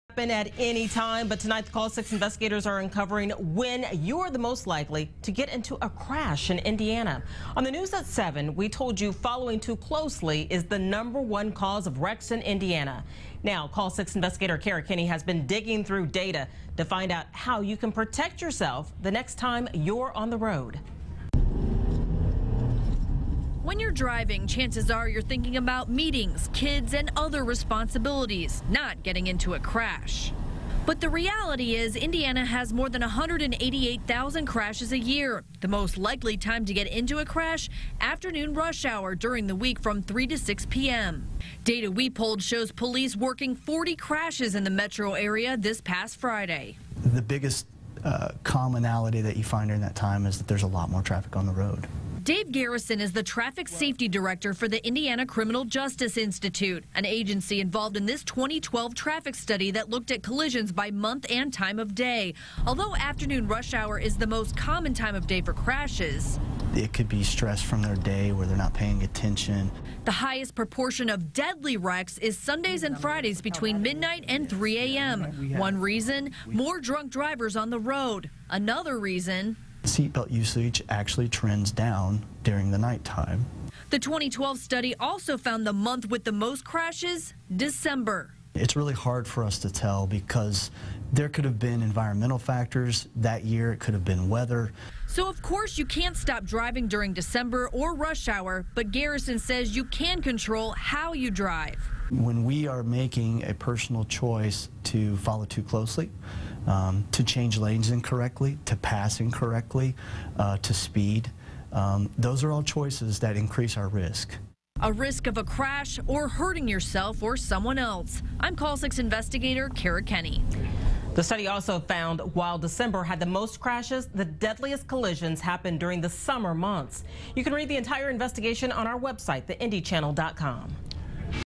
When are Indiana drivers most likely to crash?
In Indiana, you're most likely to get into a crash during the afternoon rush hour (3-5:59 p.m.) on weekdays, Call 6 Investigator Kara Kenney reported.